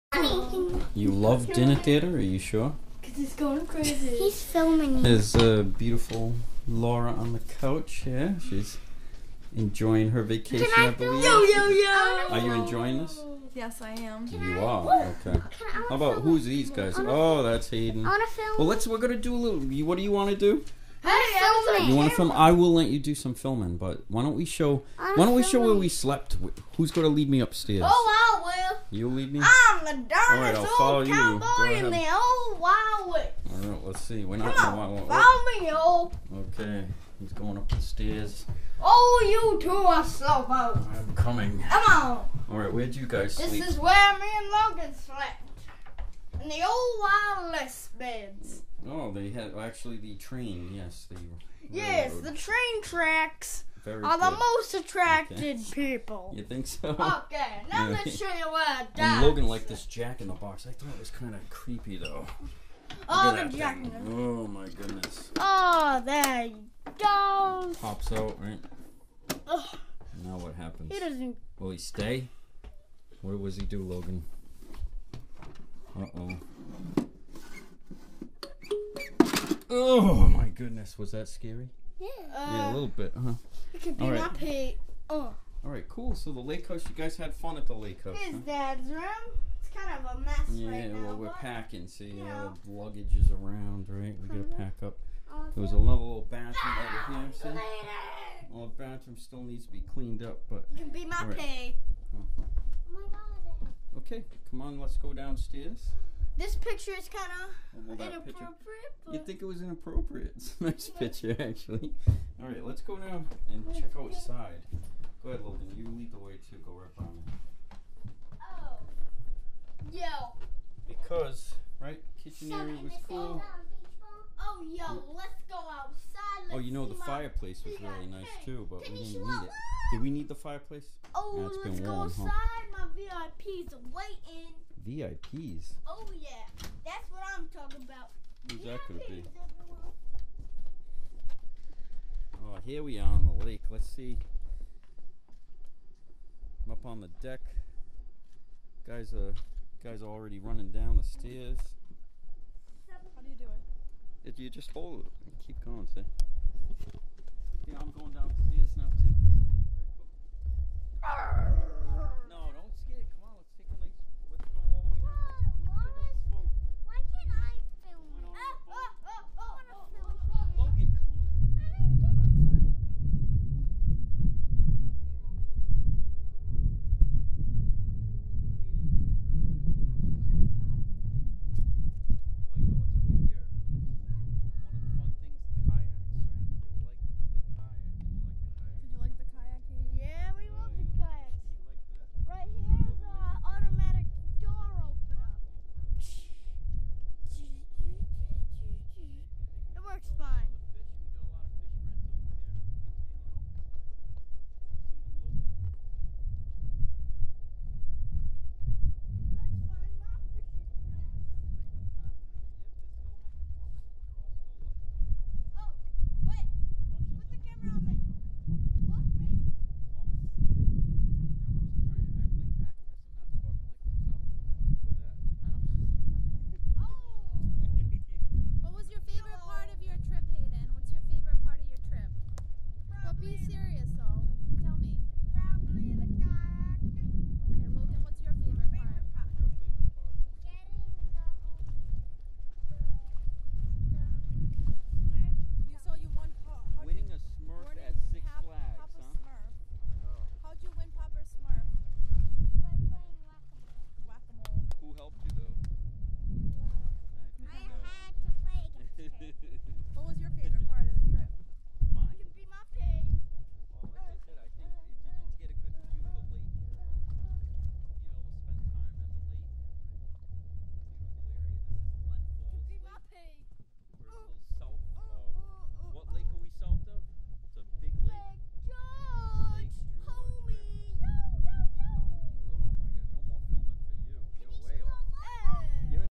Lake House Vacation Part 1 ZOOM0003.MOV
ZOOM Handy Video Recorder captures the Lake House in Glen Falls in the Lake George area, we had a great vacation thanks to Auntie Tammy and Uncle Drew!  Filmed on 8/21/12